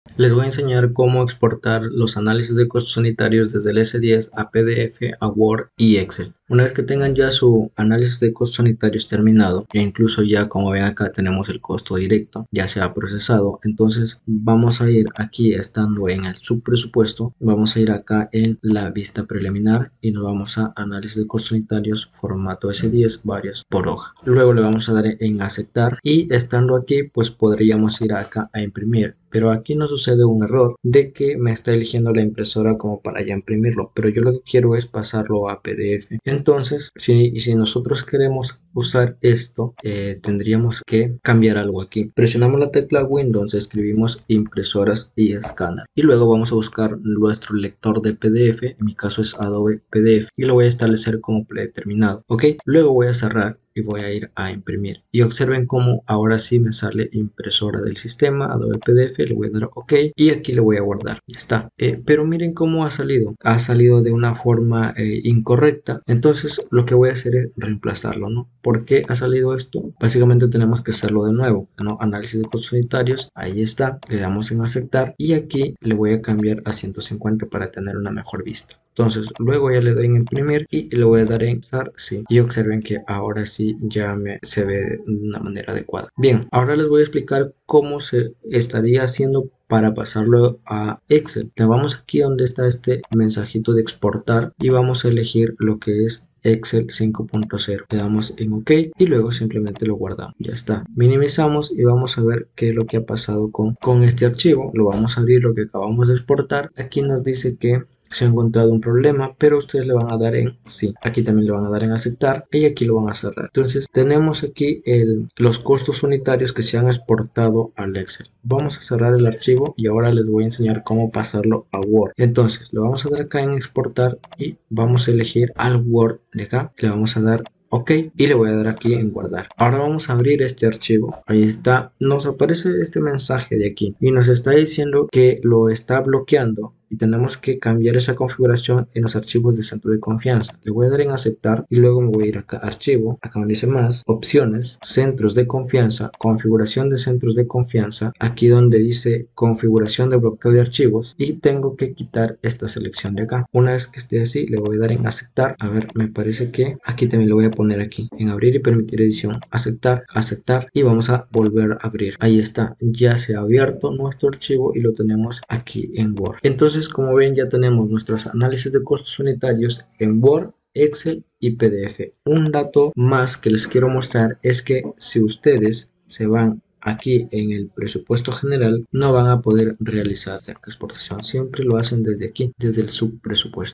EXPORTAR análisis de costos unitarios a PDF EXCEL WORD desde el S10 costos y presupuestos
¡Optimiza la comunicación de tus proyectos de construcción y comparte tus análisis de costos de manera eficiente!

En este video tutorial, te guiaremos paso a paso a través del proceso de exportar análisis de costos unitarios a PDF, Excel y Word desde tu software de gestión de proyectos. Aprenderás a:

•Generar un análisis de costos unitario completo: Obtén un desglose detallado de los costos unitarios de cada ítem en tu proyecto de construcción.
•Seleccionar el formato de exportación: Elige entre los formatos de archivo más populares, como PDF, Excel o Word, según tus necesidades específicas.
•Personalizar las opciones de exportación: Configura el aspecto del archivo exportado, incluyendo la información que se incluye, el formato del texto y el diseño general.
•Exportar el análisis de costos unitarios: Guarda el archivo exportado en tu computadora o compártelo directamente con otros interesados en el proyecto.

¿Por qué es importante exportar análisis de costos unitarios?

•Compartir información con clientes y colaboradores: Facilita la comunicación con tus clientes y colaboradores, proporcionándoles una visión clara y detallada de los costos unitarios de tu proyecto.
•Realizar análisis de costos: Exporta los datos a formatos compatibles con hojas de cálculo para realizar análisis más profundos de los costos unitarios y tomar decisiones informadas.
•Archivar registros de proyectos: Guarda copias de tus análisis de costos unitarios para referencia futura y seguimiento del rendimiento de tus proyectos.
•Cumplir con requisitos de licitación: Exporta los análisis de costos unitarios en el formato requerido para cumplir con los requisitos de licitaciones y propuestas.

¿A quién está dirigido este video?
•Constructores y contratistas que necesitan compartir análisis de costos unitarios con sus clientes, colaboradores y entidades gubernamentales.
•Ingenieros civiles y arquitectos que trabajan en proyectos de construcción y necesitan exportar análisis de costos unitarios para análisis y presentaciones.
•Estudiantes de ingeniería civil y arquitectura que aprenden a utilizar software de gestión de proyectos y herramientas de análisis de costos.

¡No te pierdas este video y aprende a exportar análisis de costos unitarios a PDF, Excel y Word como un profesional!
¡No te arrepentirás!